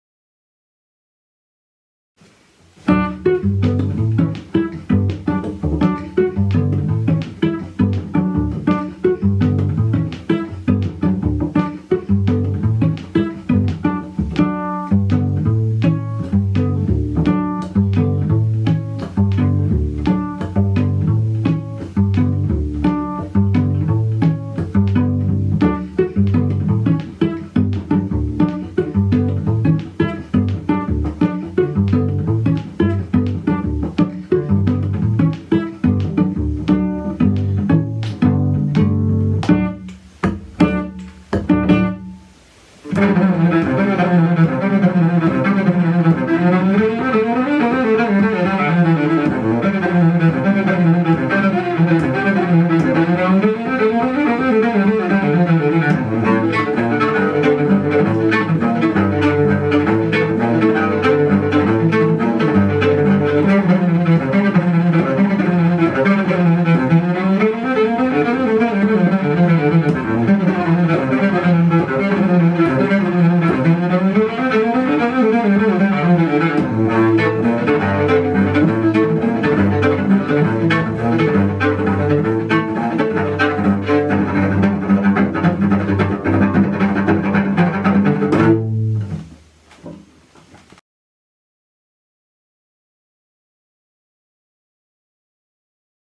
RestezChezVous avec Vincent Segal et le Festival International de Violoncelle de Beauvais
Vincent Segal, depuis son confinement, offre son moment musical à l'Association pour le Rayonnement du Violoncelle. Structure organisatrice du Festival International de Violoncelle de Beauvais.